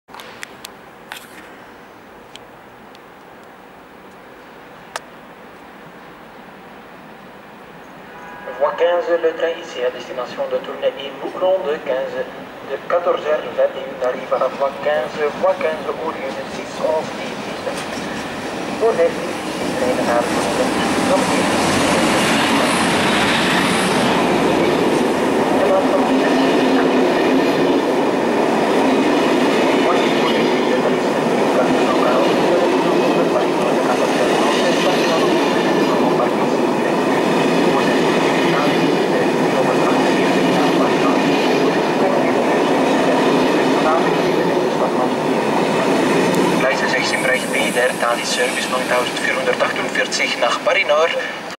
HLE 2153 + M4 Bruxelles-Midi
Arrivée à Bruxelles-Midi de la 2153 et des M4 sur l'IC-H 1935 pour Mouscron.
Aankomst aan Brussel-Zuid van de 2153 met M4's op de IC-H 1935 voor Moeskroen.
08-03-14